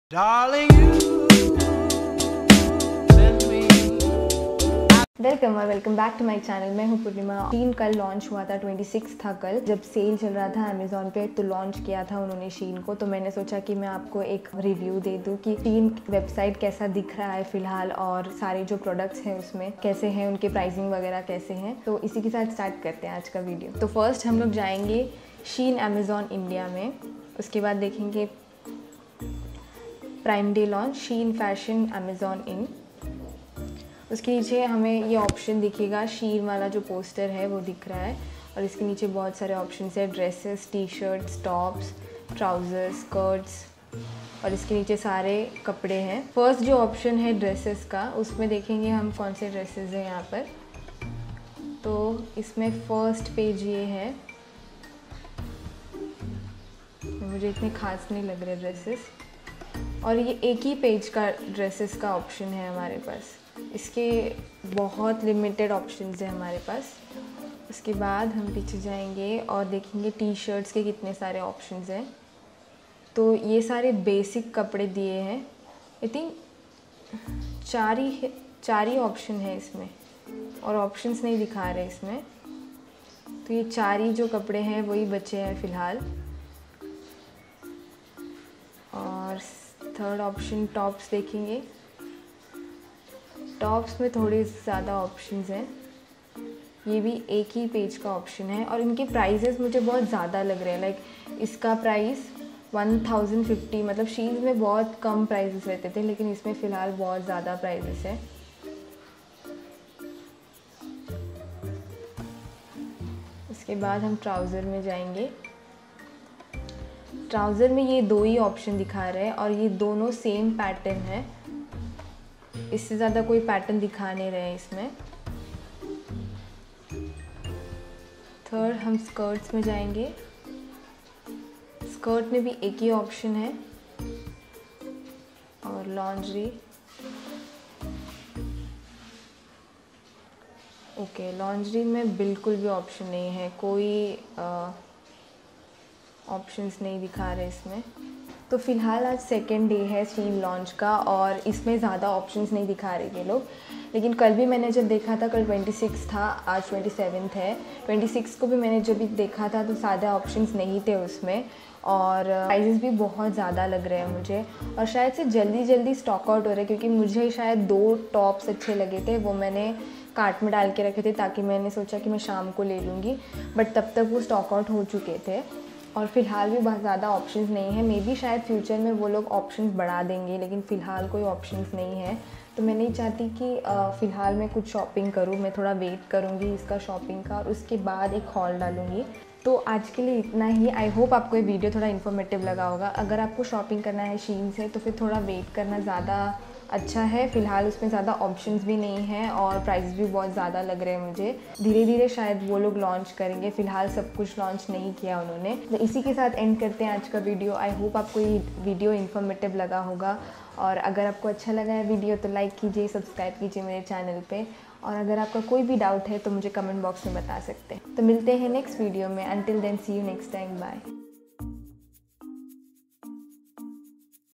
SHEIN IS BACK I SHEIN REVIEW VIDEO
Shein has launched in India recently through Amazon. I would like to review it. just a quick video to update you all the products.